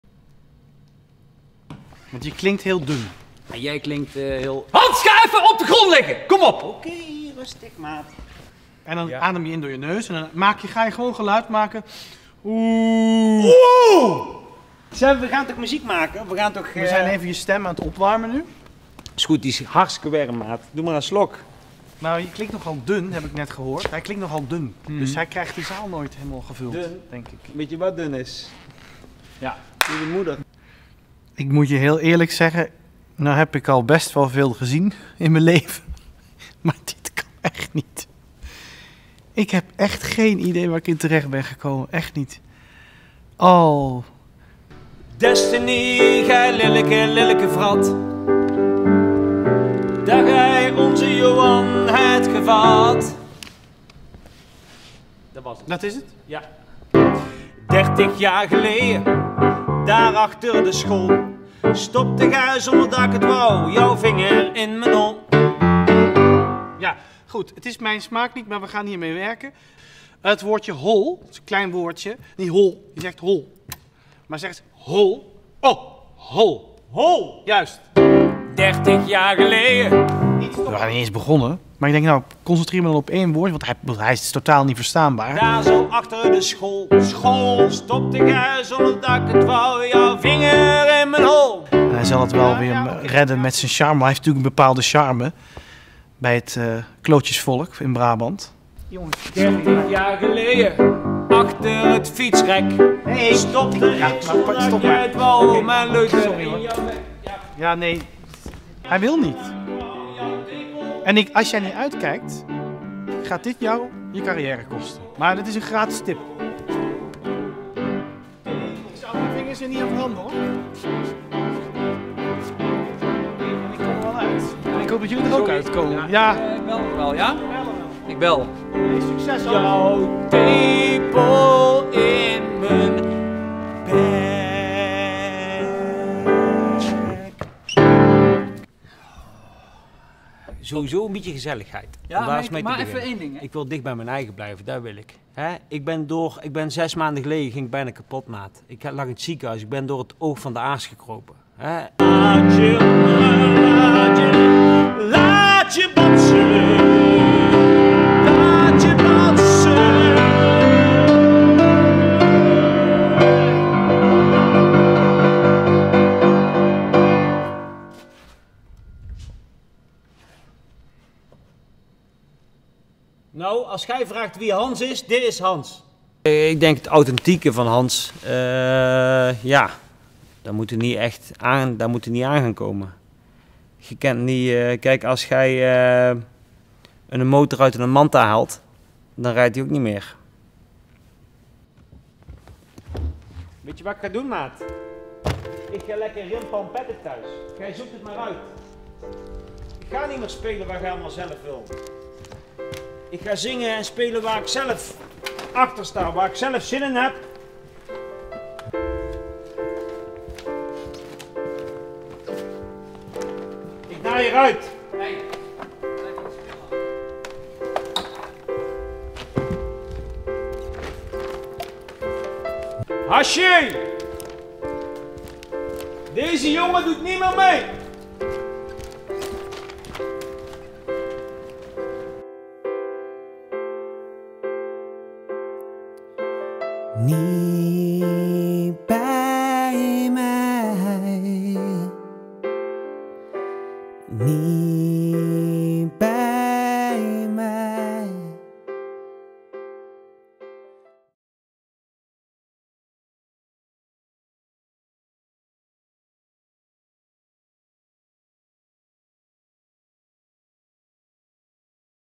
Hans gaat voor goud | Aflevering 6
De laatste aflevering van de mini docu van Zanger Hans. Of zal er dan toch morgen een nieuwe single komen?